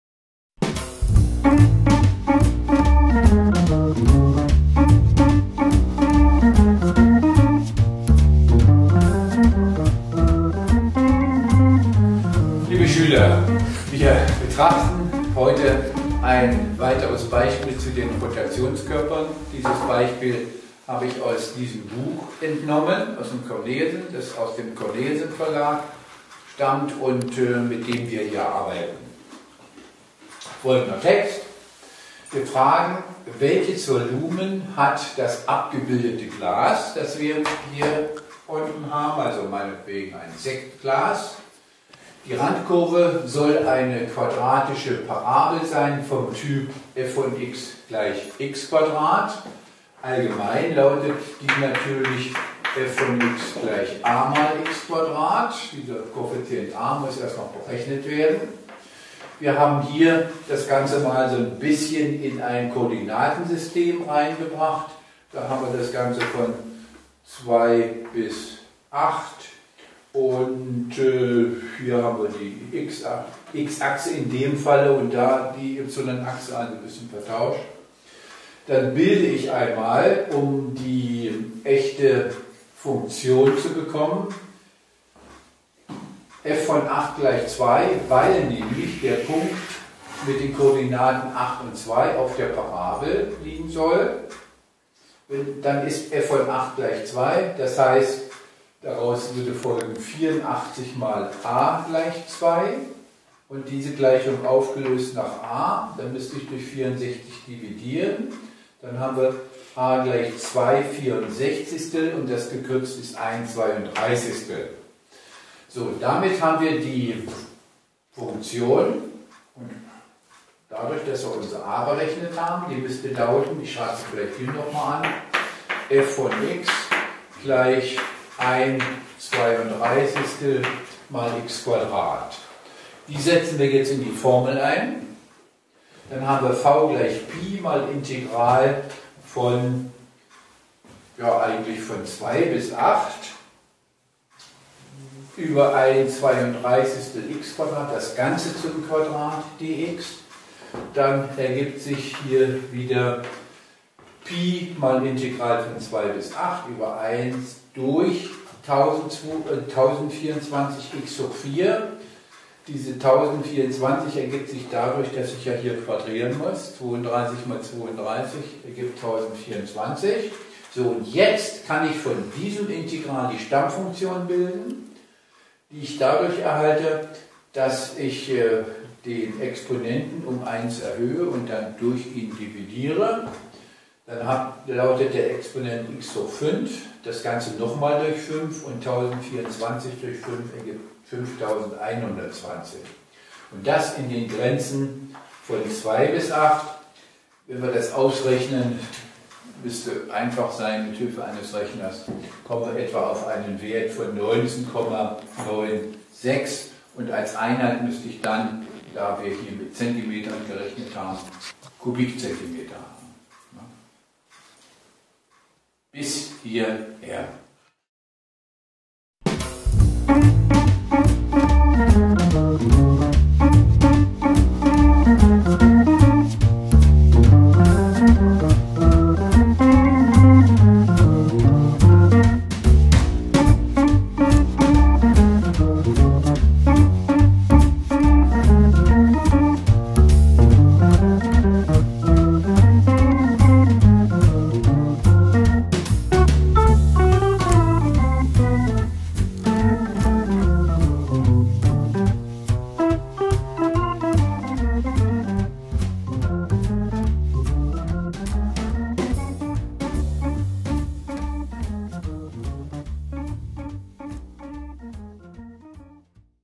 Berechnung von Rotationskörpern
Es wird das Volumen eines Sektglases ermittelt.